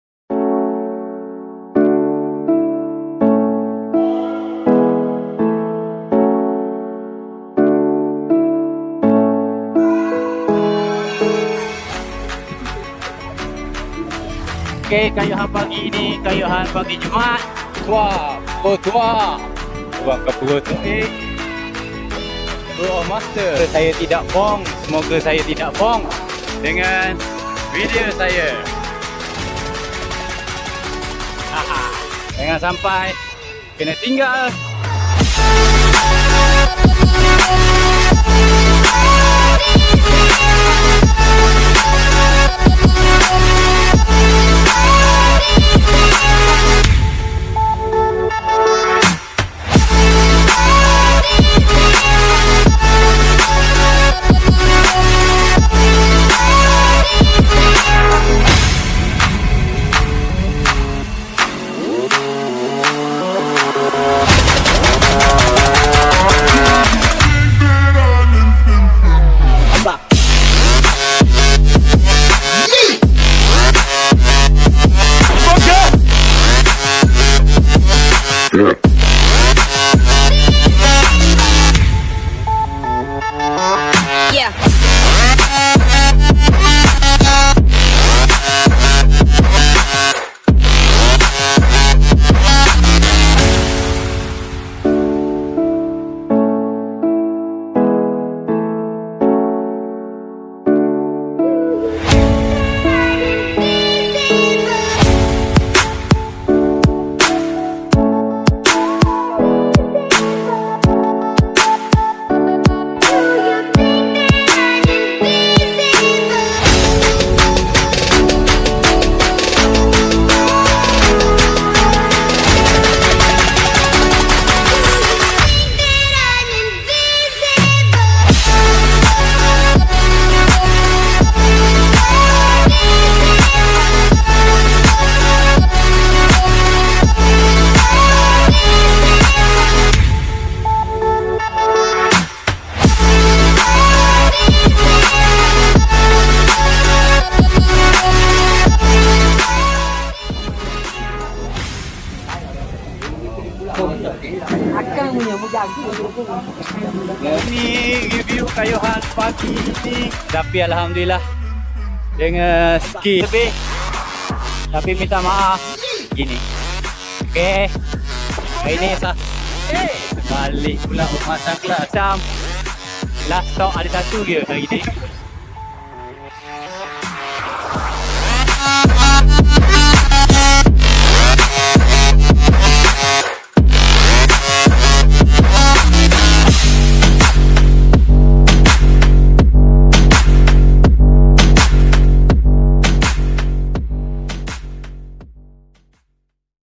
Go8 Friday Ride Terengganu
Kayuhan gabungan semua kelab2 di Ganu Kita.. Jom2 join kami setiap pagi jumaat